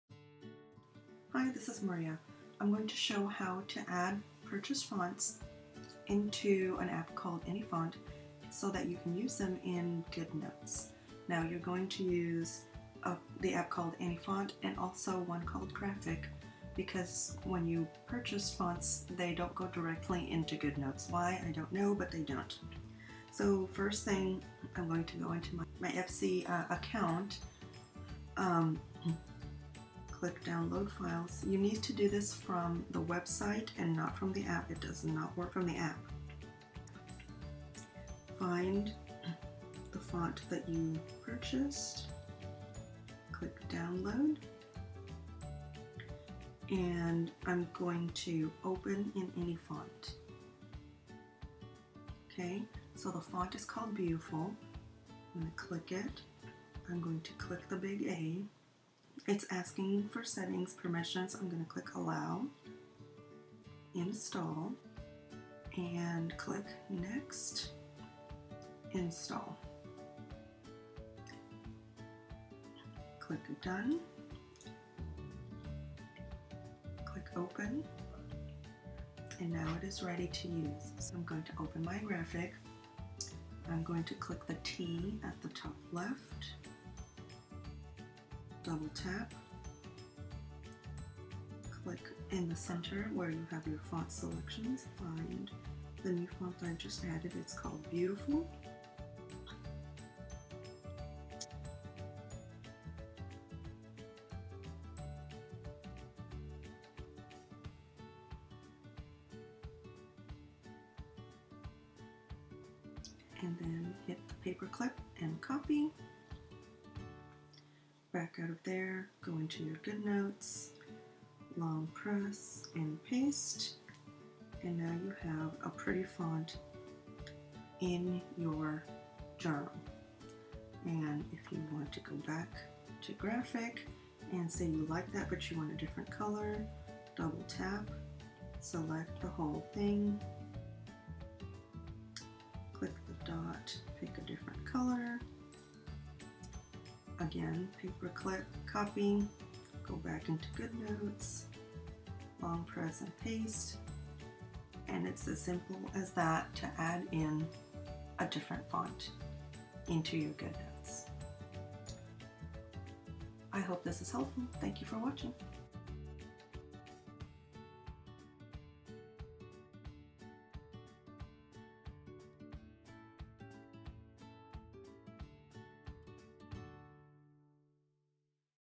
Prchased fonts using Goodnotes and Graphic.
How to load new fonts with Anyfont and use them in Graphic, copy/paste into Goodnotes.